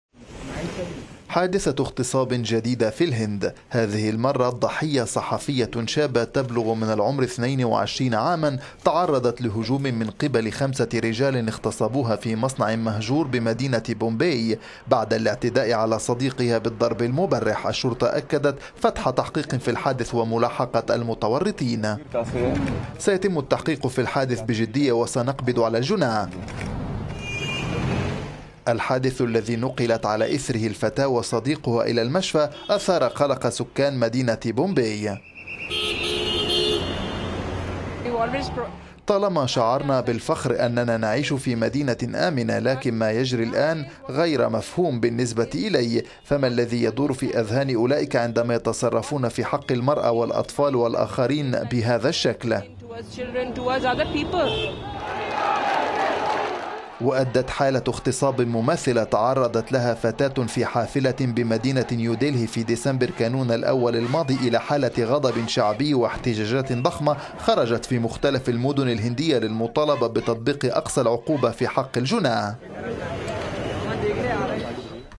اغتصاب جماعي لصحفية في الهند
الهند تستيقظ على حادثة اغتصاب جديدة، هذه المرة الضحية...

الهند تستيقظ على حادثة اغتصاب جديدة، هذه المرة الضحية صحفية شابة تبلغ من العمر اثنين وعشرين عاما تعرضت لهجوم من قبل خمسة رجال اغتصبوها في مصنع مهجور بمدينة بومباي، بعد الاعتداء على صديقها بالضرب المبرح. الشرطة أكدت من جهتها فتح تحقيق في الحادث وملاحقة المتورطين.

الحادث الذي نقلت على إثره الفتاة وصديقها إلى المشفى اثار قلق سكان مدينة بومباي.

فتاة هندية من مدينة بومباي:
"طالما شعرنا بالفخر أننا نعيش في مدينة أمنة. لكن ما يجري الان غير مفهوم بالنسبة إلي. فما الذي يدور في ذهن أولئك عندما يتصرفون في حق المراة والاطفال والاخرين بهذا الشكل."